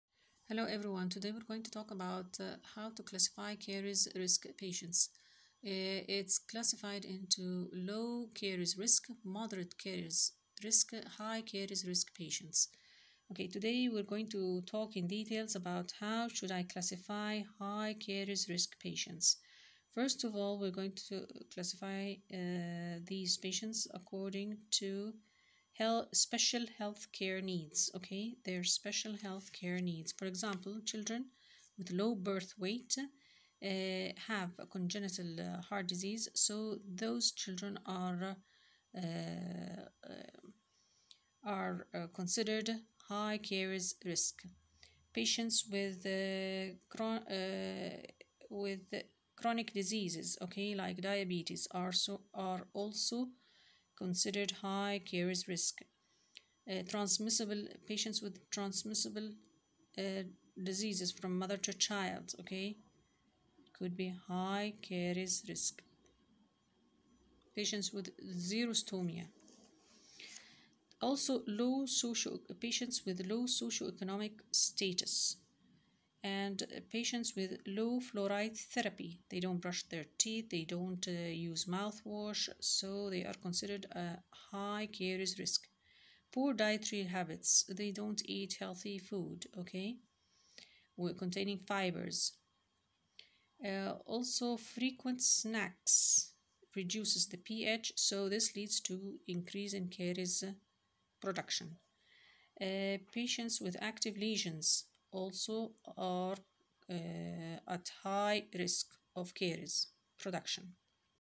129- How to classify the high caries risk patients?
Today we talked about how to classify high caries risk patients.